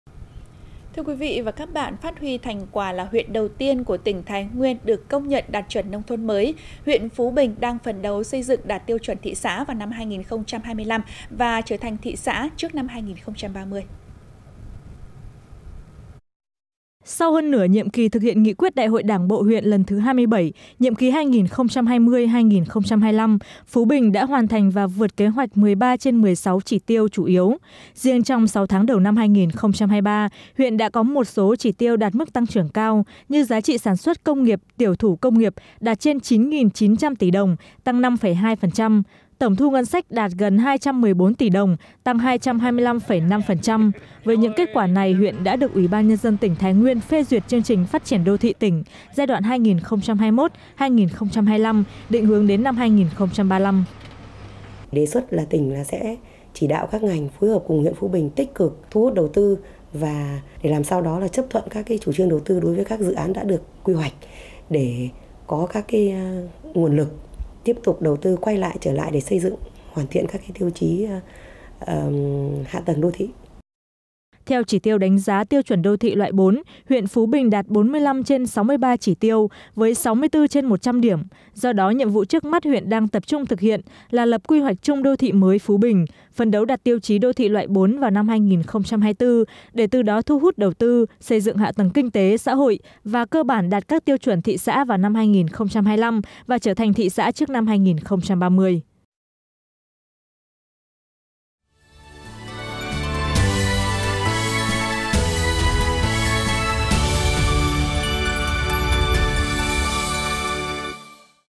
Thái Nguyên: Huyện Phú Bình phấn đấu đạt tiêu chuẩn thị xã vào năm 2025
Thái Nguyên: Huyện Phú Bình phấn đấu đạt tiêu chuẩn thị xã vào năm 2025 | Truyền Hình Nhân Dân

Phát huy thành quả là huyện đầu tiên của tỉnh Thái Nguyên được công nhận đạt chuẩn nông thôn mới, huyện Phú Bình đang phấn đấu xây dựng đạt tiêu chuẩn thị xã vào năm 2025 và trở thành thị xã trước năm 2030.

Sau hơn nửa nhiệm kỳ thực hiện Nghị quyết Đại hội Đảng bộ huyện lần thứ 27, nhiệm kỳ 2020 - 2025, Phú Bình đã hoàn thành và vượt kế hoạch 13/16 chỉ tiêu chủ yếu. Riêng trong 6 tháng đầu năm 2023, huyện đã có một số chỉ tiêu đạt mức tăng trưởng cao, như giá trị sản xuất công nghiệp - tiểu thủ công nghiệp đạt trên 9.900 tỷ đồng (tăng 5,2%); tổng thu ngân sách đạt gần 214 tỷ đồng (tăng 225,5%); Với những kết quả này, huyện đã được UBND tỉnh Thái Nguyên phê duyệt chương trình phát triển đô thị tỉnh giai đoạn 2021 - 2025, định hướng đến năm 2035

PV1: Đồng chí Nguyễn Thị Loan – Bí thư Huyện uỷ, Chủ tịch UBND huyện Phú Bình, tỉnh Thái Nguyên
 (Theo quy hoạch tỉnh, quy hoạch vùng huyện được phê duyệt, hiện nay huyện xác định được các khu cụm công nghiệp, dự án nghĩa trang tập trung, nhà máy xử lý môi trường công nghệ cao, các dự án về khu dân cư khu đô thị.Huyện cũng đề xuất tỉnh chỉ đạo các ngành phối hợp cùng huyện tích cực thu hút đầu tư, chấp thuận phê duyệt với các dự án đã được quy hoạch để có các nguồn lực tiếp tục đầu tư quay lại để xây dựng hoàn thiện các tiêu chí hạ tầng đô thị).

Theo chỉ tiêu đánh giá tiêu chuẩn đô thị loại 4, huyện Phú Bình đạt 45/63 chỉ tiêu với 64/100 điểm. Do đó, nhiệm vụ trước mắt huyện đang tập trung thực hiện là lập quy hoạch chung đô thị mới Phú Bình, phấn đấu đạt tiêu chí đô thị loại 4 vào năm 2024 để từ đó thu hút đầu tư, xây dựng hạ tầng kinh tế, xã hội và cơ bản đạt các tiêu chuẩn thị xã vào năm 2025 và trở thành thị xã trước năm 2030.
1. Dự Báo Thời Tiết Hôm Nay